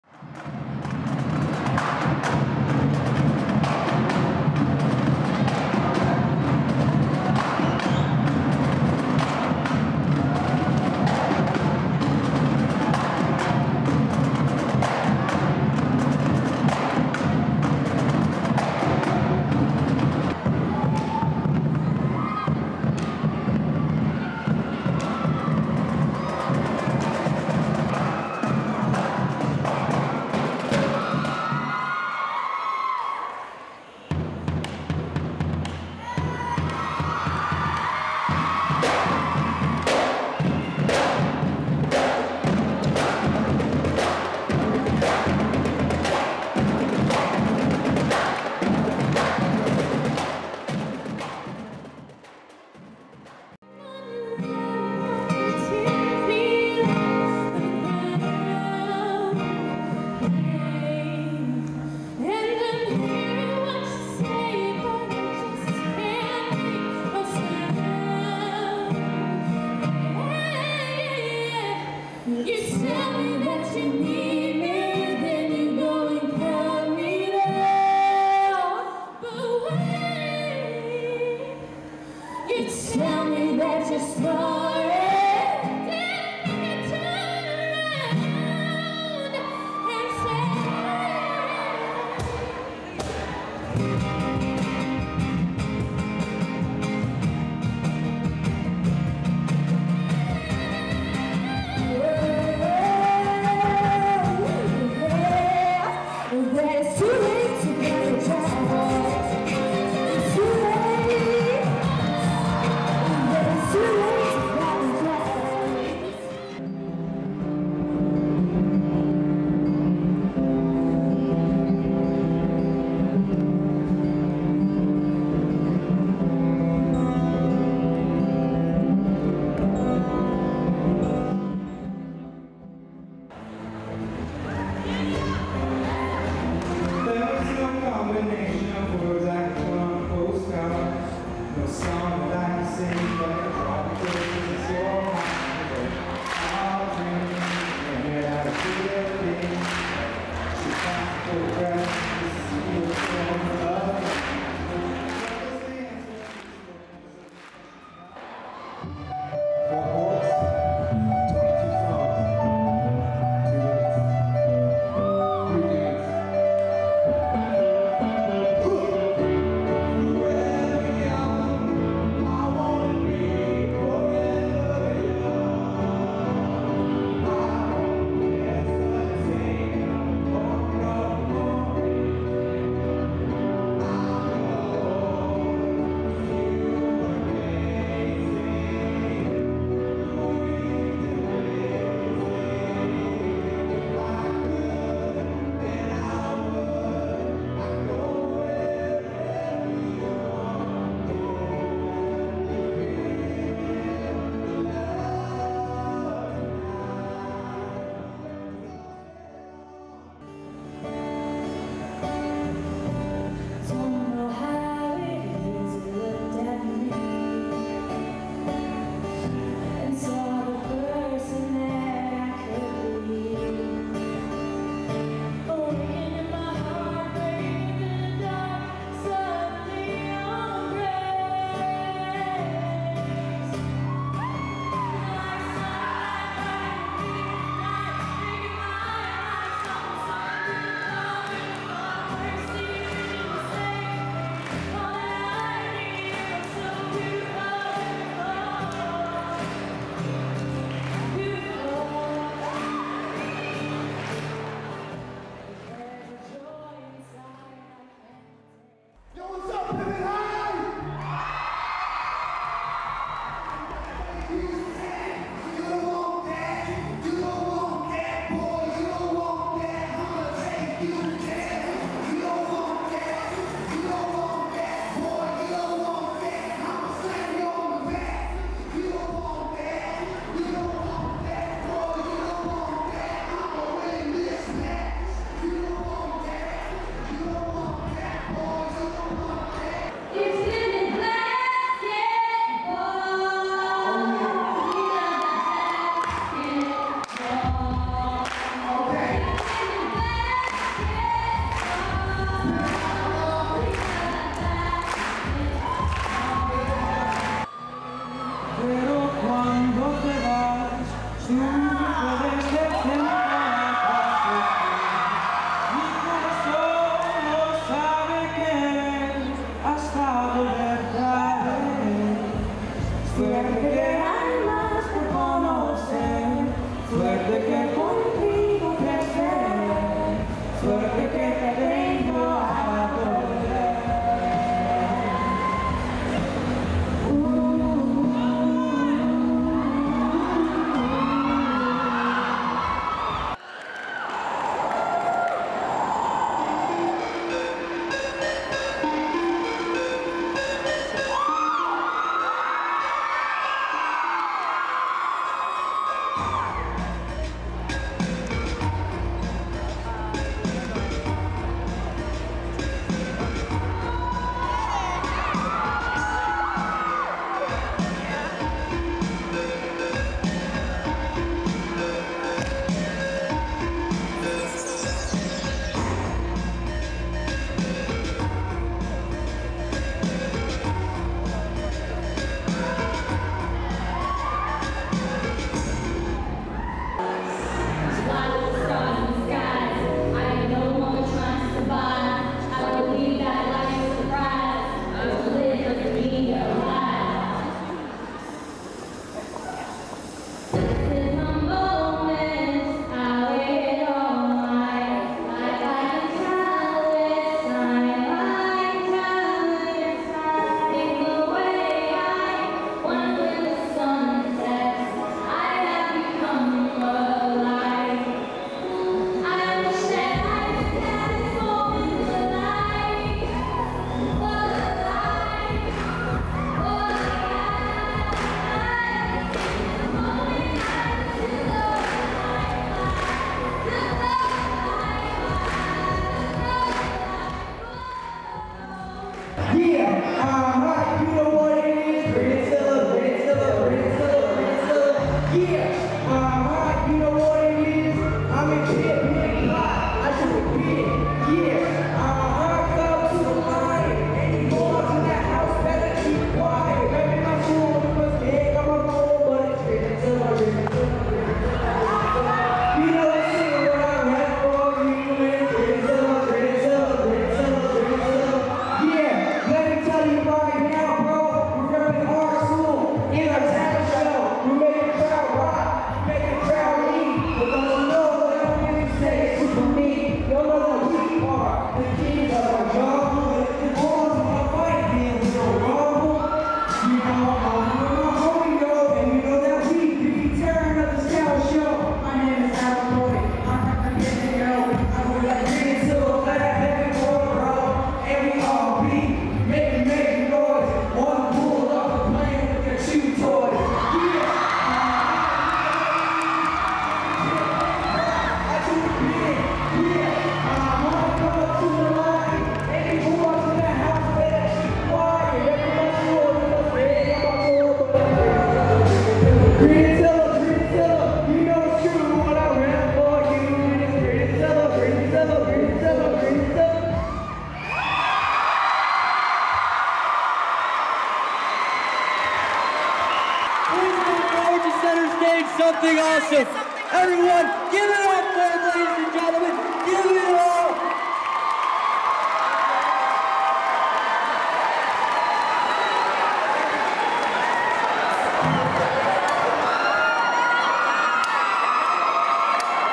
Highlights from the "Pitman's Got Talent" Rally!
These are some highlights from Pitman High School's "Pitman's Got Talent" Rally from February 2011!